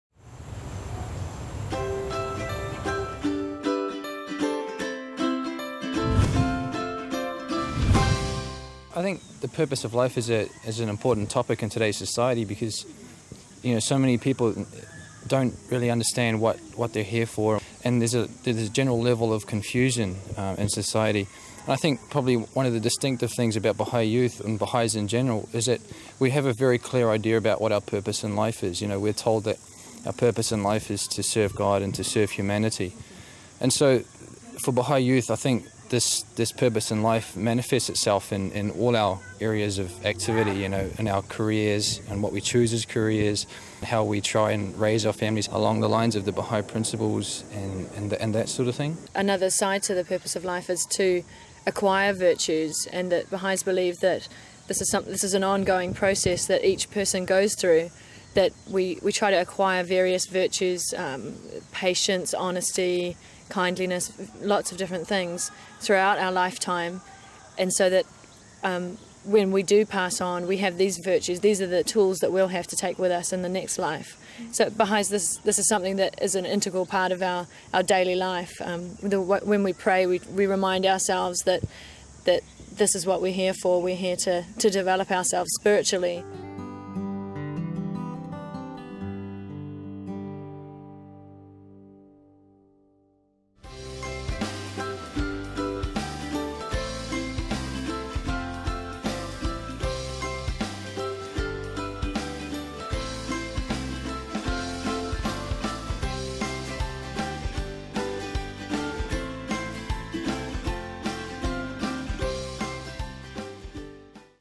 New Zealand Baha'i Youth-Bahá'u'lláh's Teaching on the Purpose of Life
A group of New Zealand Bahá’í youth discuss Baha'u'llah's Holy Writings on why were we created. What is the purpose of life and developing virtues.